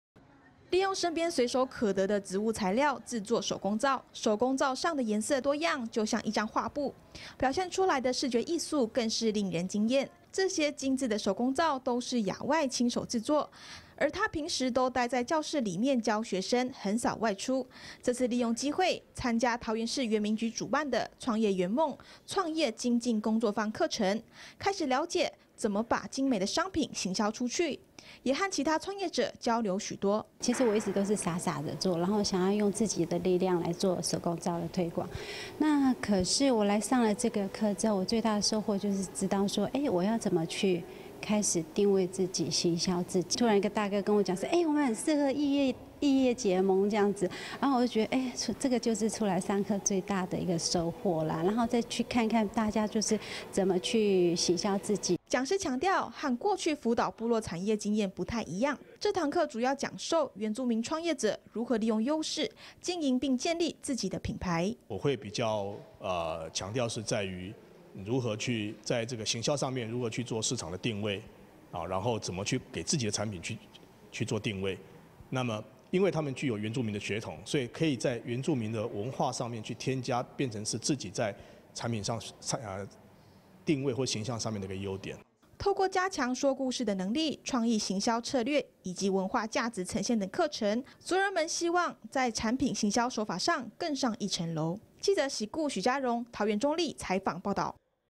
桃市"創業原夢"開課 教產品行銷手法 2017-11-18 TITV 原視新聞
利用身邊隨手可得的植物材料，製作手工皂，手工皂上的顏色多樣，就像一張畫布，表現出來的視覺藝術，更是令人驚艷。

這些精緻的手工皂，都是Yawai親手製作，而她平時都待在教室裡面教學生，很少外出，這次利用機會參加桃園市原民局主辦的「創業原夢－創業精進工作坊」課程，開始了解怎麼把精美的商品行銷出去，也和其他創業者交流許多。

講師強調，和過去輔導部落產業經驗不太一樣，這堂課主要講授原住民創業者如何利用優勢，經營並且建立自己的品牌。

透過加強說故事能力、創意行銷策略以及文化價值呈現等課程，族人們希望在產品行銷手法更上一層樓。